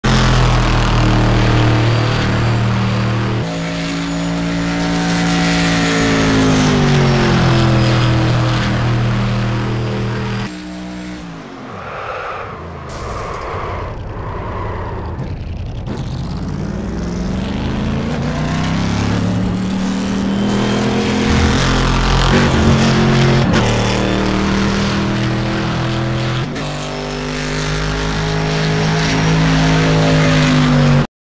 Corvette at Daytona
Maybe a bit too much rear brake bias? Definitely not how to get through the bus stop at Daytona.